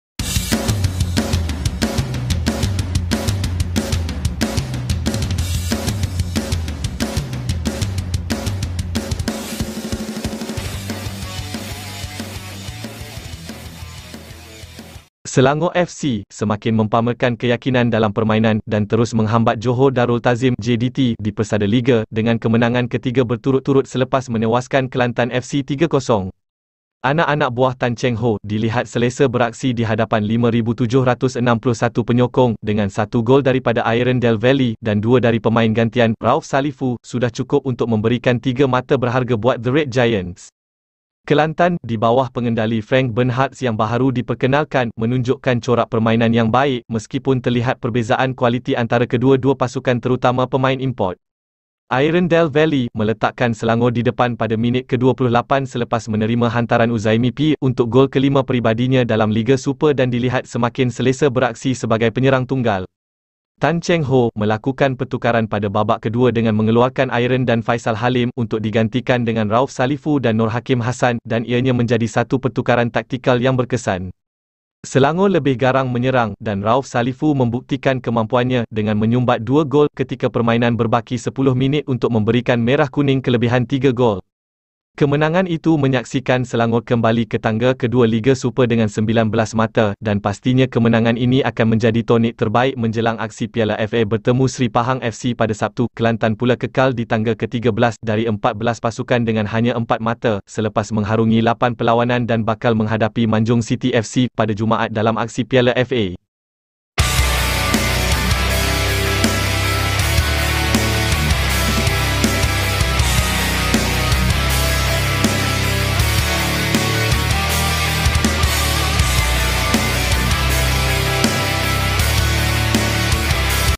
SELANGOR FC KEMENANGAN TIGA KALI BERTURUT²
Anak-anak buah Tan Cheng Hoe, dilihat selesa beraksi dihadapan 5,761 penyokong, dengan satu gol daripada Ayron Del Valle, dan dua dari pemain gantian, Rauf Salifu, sudah cukup untuk memberikan tiga mata berharga buat The Red Giants.